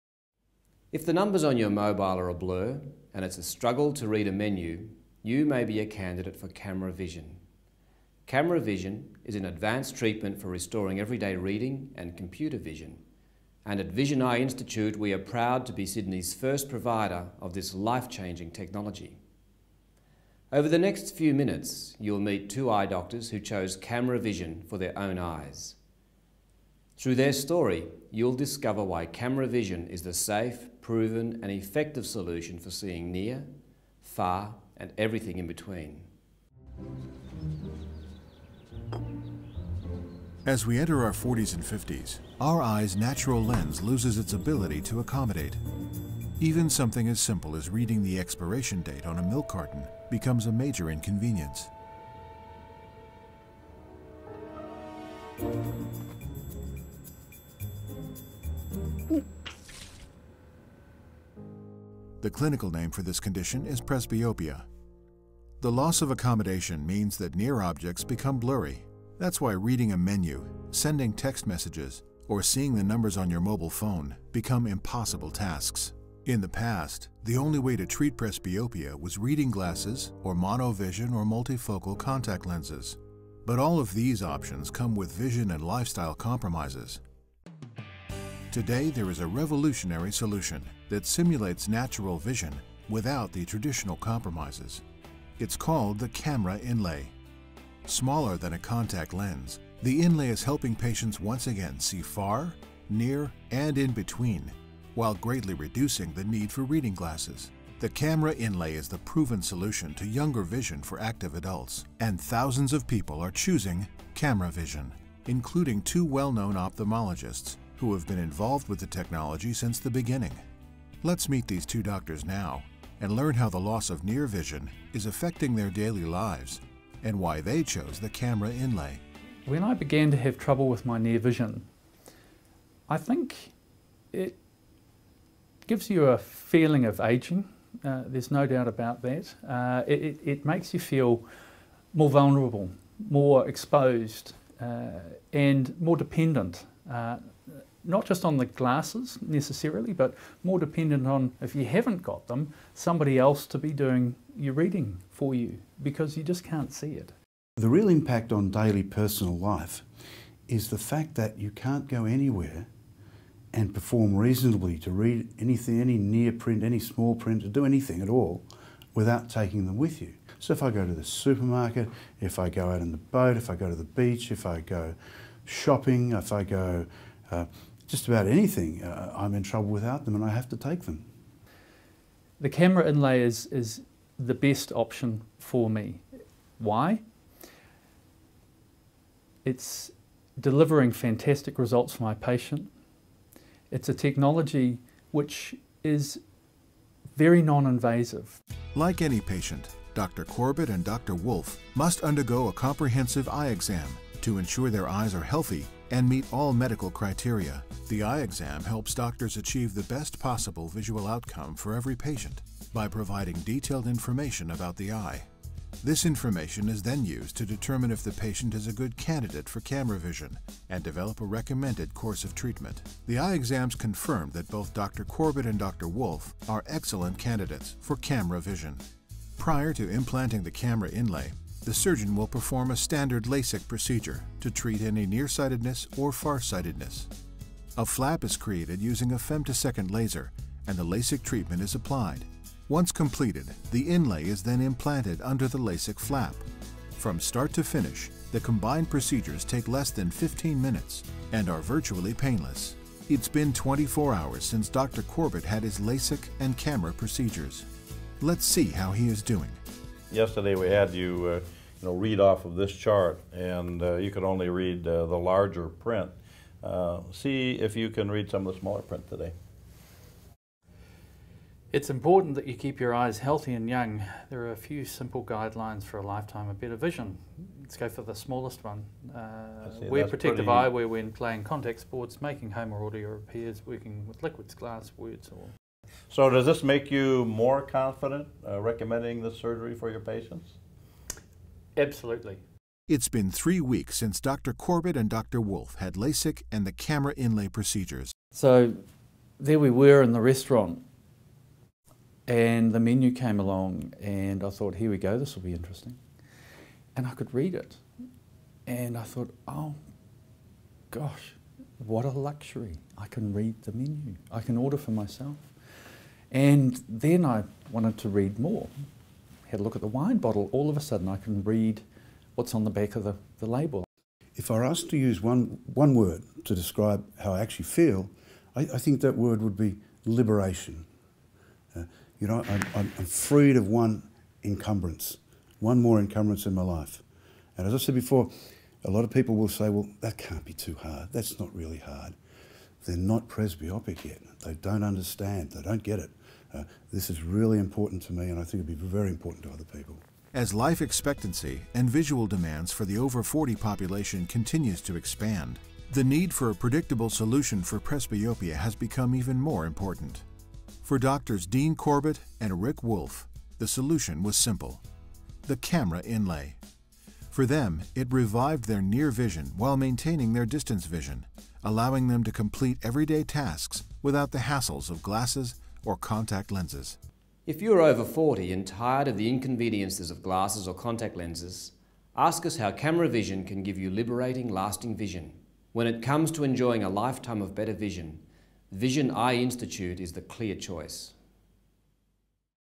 KAMRA by Acufocus - Dr Patrick Versace
Dr Patrick Versace introduces a video which explains Kamra by Acufocus, a corneal inlay used as a procedure to correct age-related presbyopia.